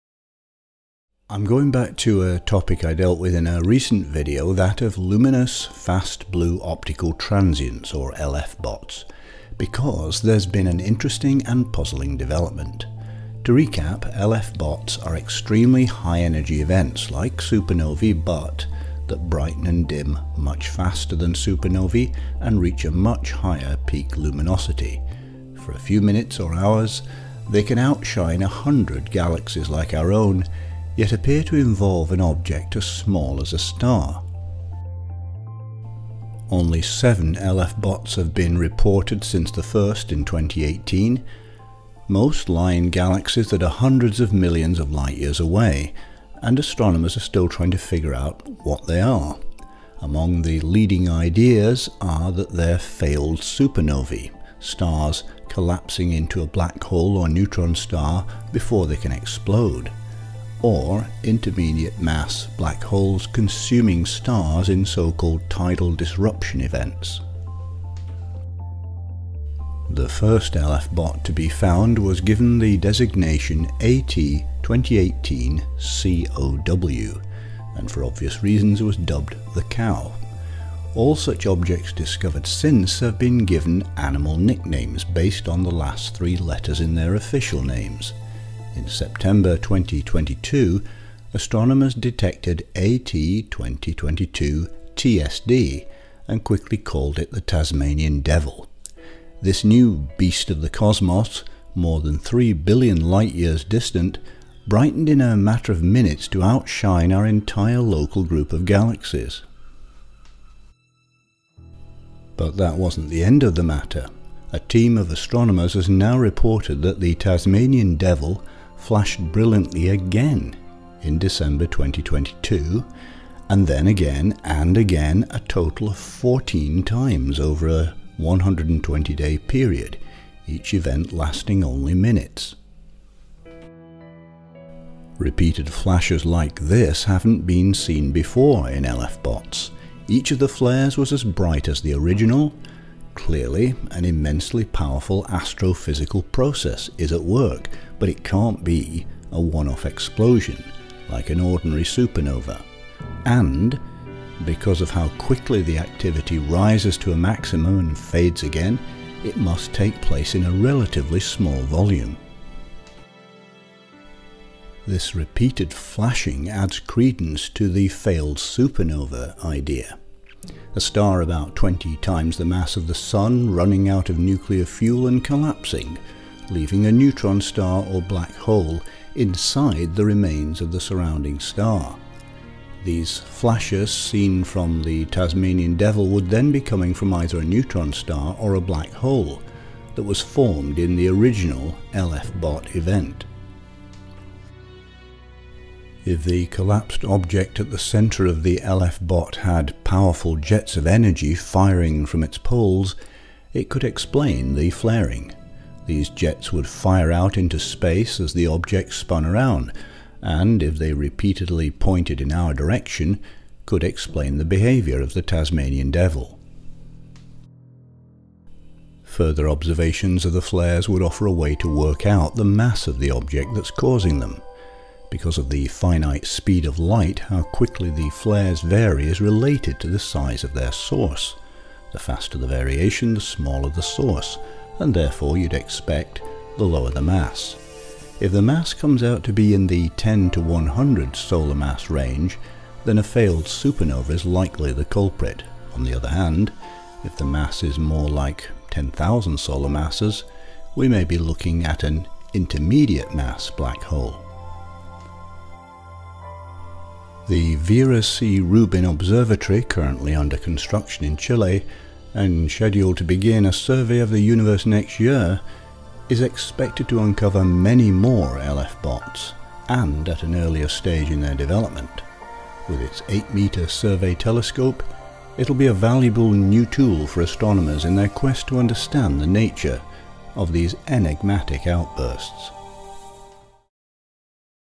The Tasmanian Devil: surprising behaviour of a luminous fast blue optical transient
A team of astronomers has reported that an LFBOT known as the Tasmanian Devil flashed brilliantly again in December 2022 and then again and again – a total of 14 times over a 120-day period, each event lasting only minutes.
 Repeated flashes like this haven’t been seen before in LFBOTs.